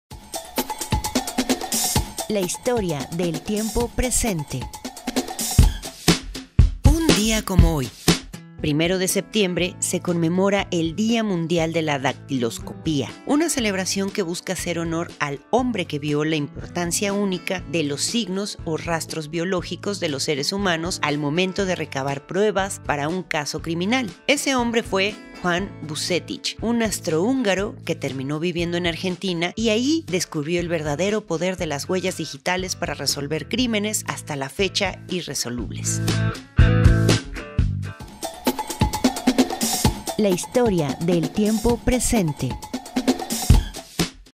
Día mundial de la Dactiloscopía
👍🏻👍🏼👍🏽👍🏾👍🏿Juan Vucetich desarrolló la primera clasificación de las huellas dactilares y género las primeras fichas dactiloscópicas del mundo.
Vucetich creó 4 grandes grupos para clasificar los rasgos de una huella: arcos, presillas internas, presillas externas y verticilos ✋

Sintoniza el 📺18.1 TV abierta y 118 en Megacable para más contenido aquí en TVBUAP La imagen de la Universidad